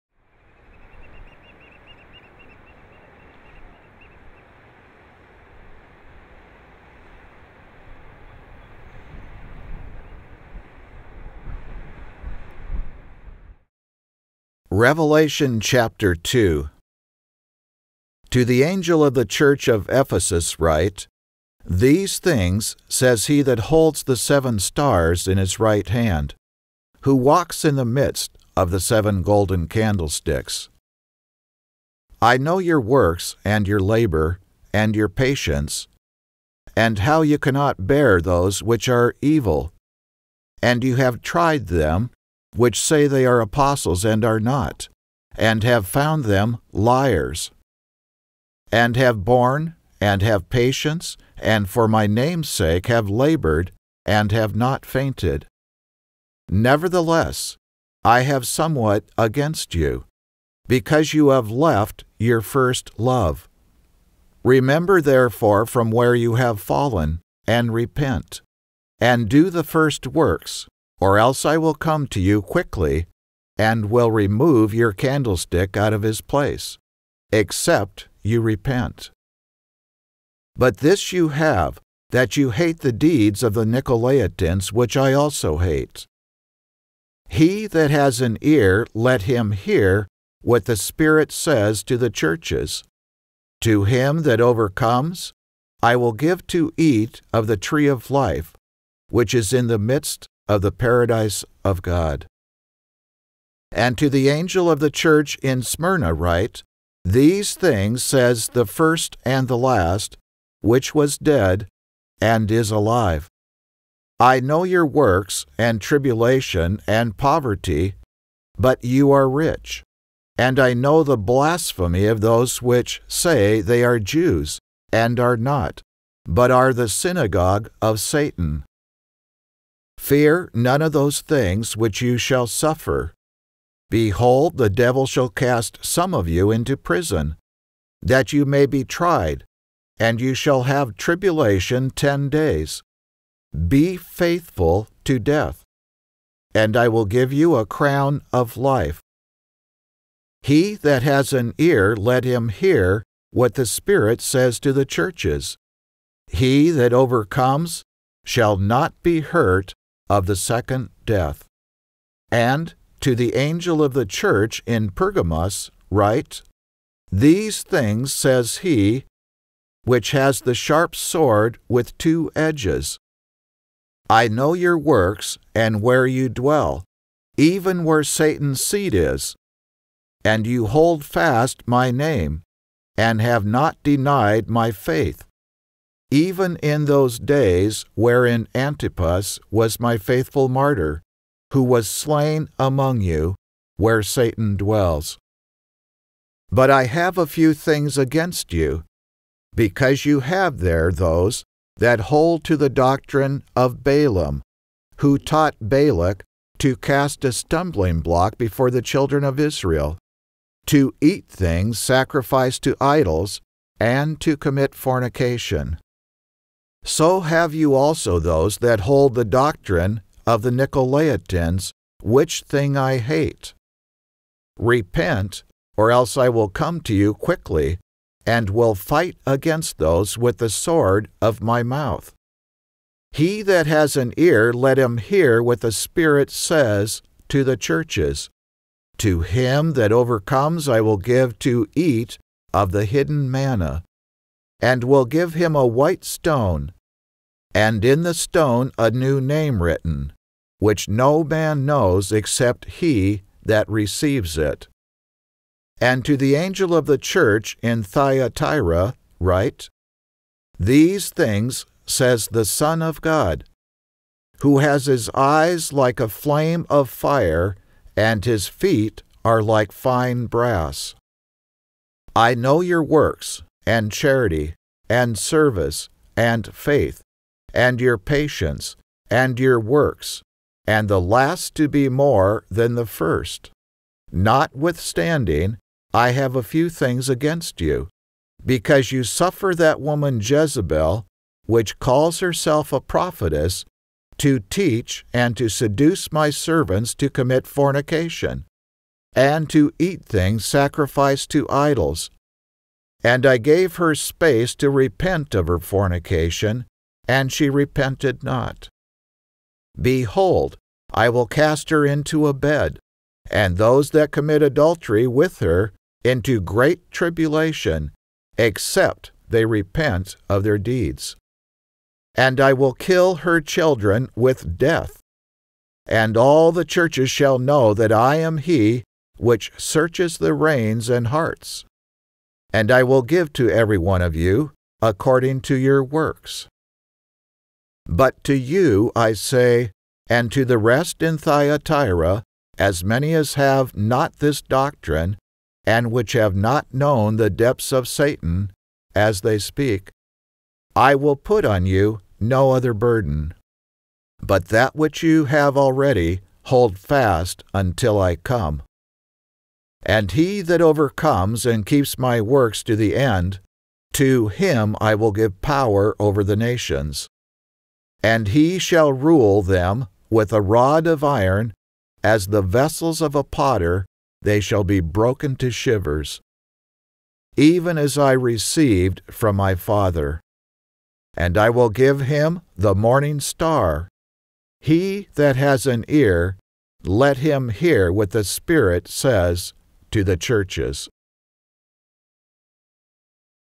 Revelation Chapter 2
A narration of the Book of Revelation using a close paraphrase of theKing James Version. Text is included for each verse. The intro video isfrom the Oregon coast. It is perhaps like the island of Patmos where theapostle John received the vision of Jesus Christ. These are things which willhappen on the earth in the days ahead.Am I prepared to face the God of heaven and earth? Even John who knew Jesus on the earth fell as a "dead man" at his feet when John came into the presence of Jesus Christ in the heavenly vision. Accepting and believing that Jesus paid the price for our sins with his blood will prepare us for our personal encounter with the living God. God loves us with a passion.